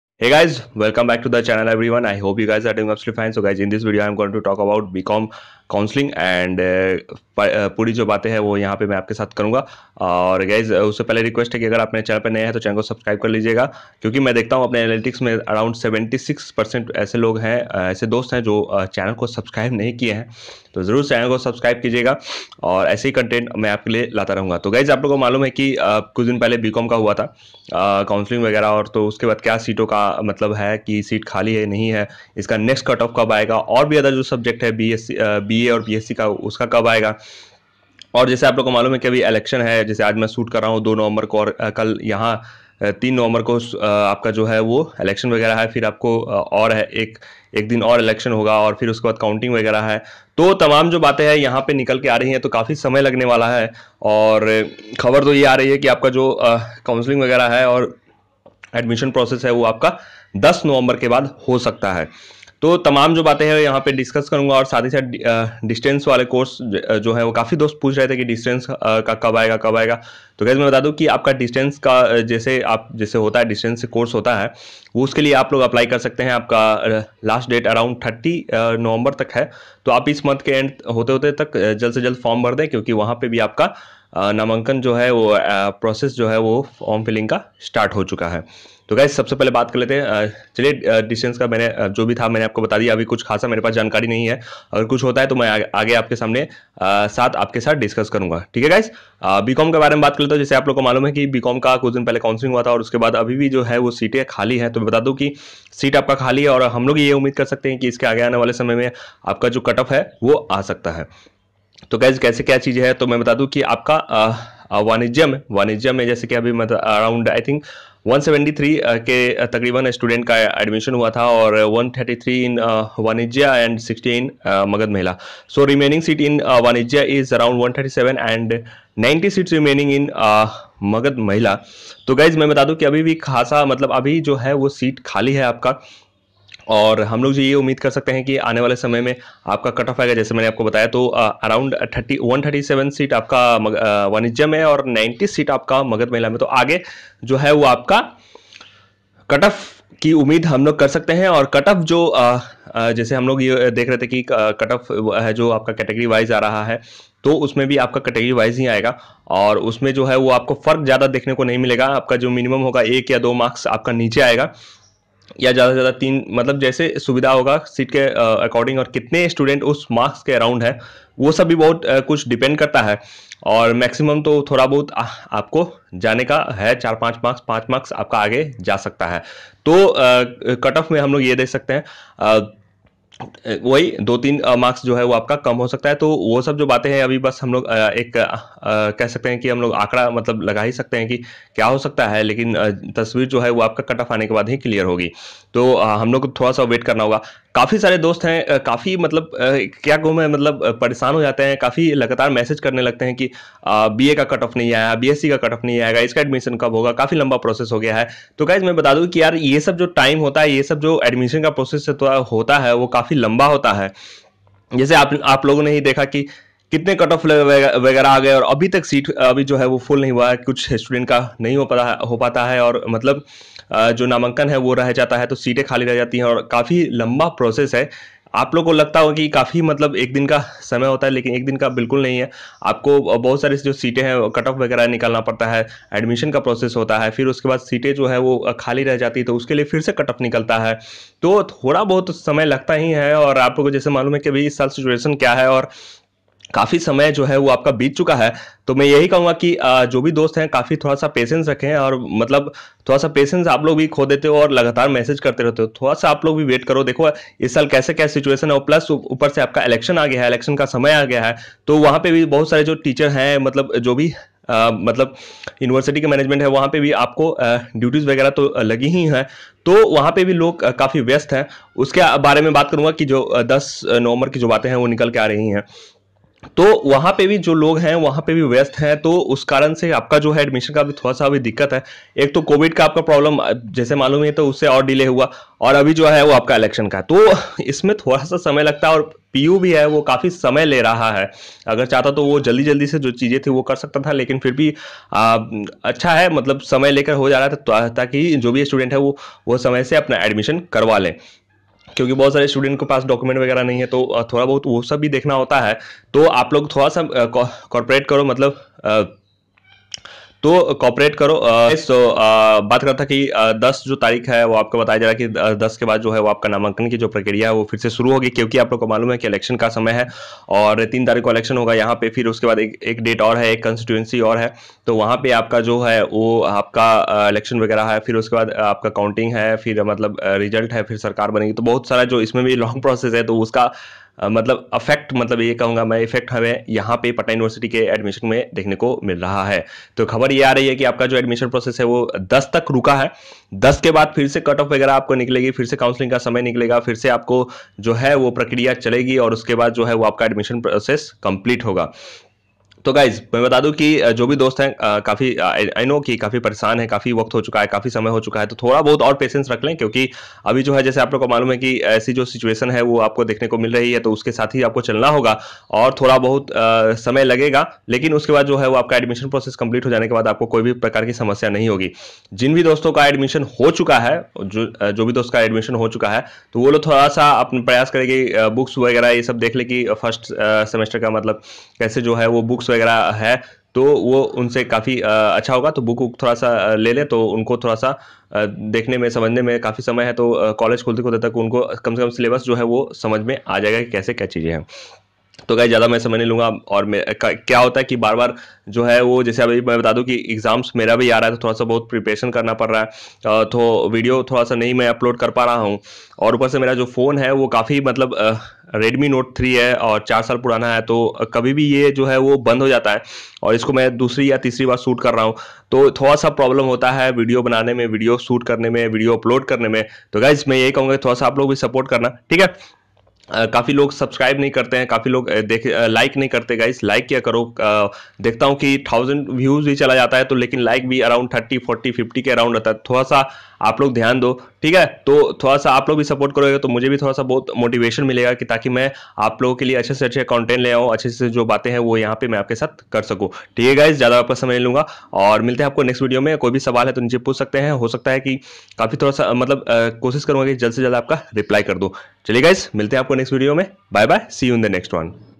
B.COM SEAT ANALYSIS AFTER COUNSELLING / DDE PATNA UNIVERSITY/ NEXT CUT OFF DATE
Hello Guys Welcome To The Video In This Video I Talked About Many Things, B.COM Counselling And Seat Remaining And Can Next Cut Off Will Declare Or Not Then About DDE And Then About Next Cut Off Date, Means When It About To Come So, Watch The Full Video.

Help Me To Improve My Content Suggest Any Video You Want To See!

This Is AkrVlogs And I Make Vlogs And Informative Video.